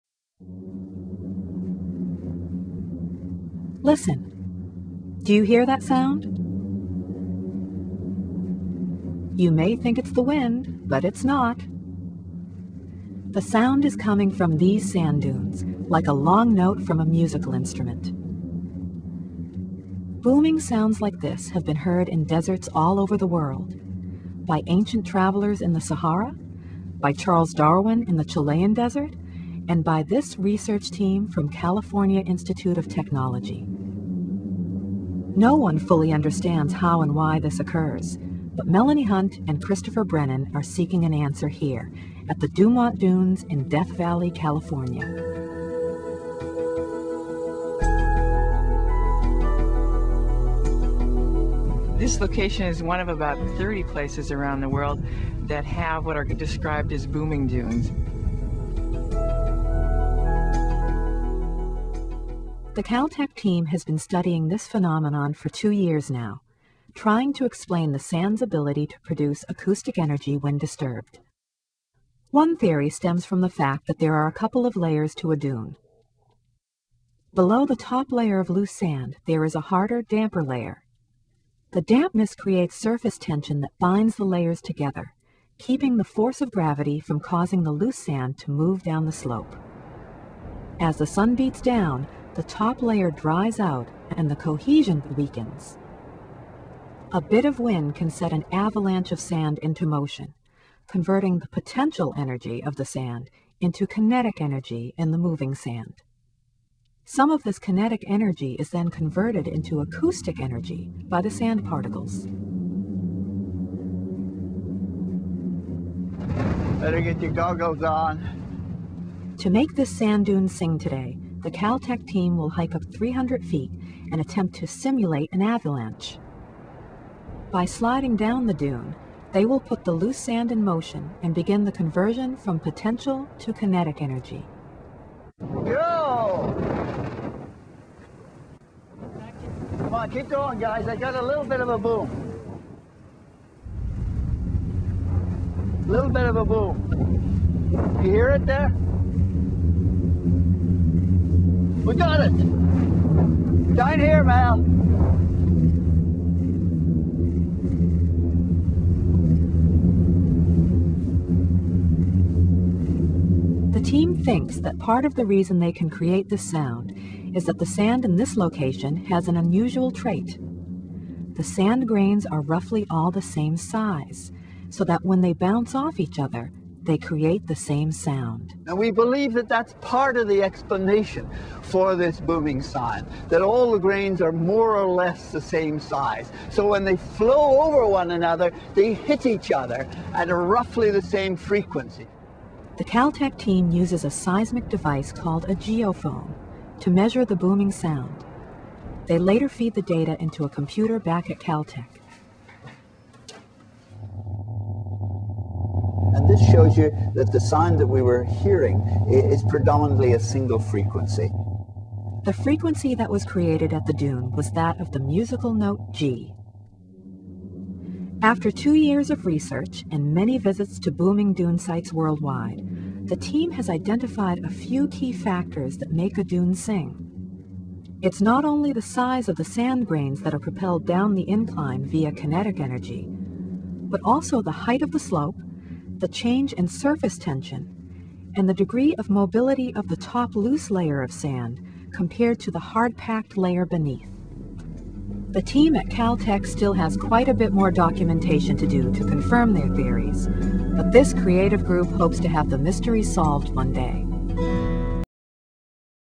Booming Sands
Sand on a dune possesses energy in the form of gravitational potential energy. The greater the height of the sand dune, the more gravitational potential energy the sand on top has. When the sand on top of a steep dune is disturbed, it slides down the slope, and gravitational potential energy is transformed into kinetic energy. It is this kinetic energy of the falling sand that produces the booming sound. As the sand particles bounce against each other, some of their kinetic energy is transformed into acoustic energy -- or sound, waves of vibrations transmitted through a medium.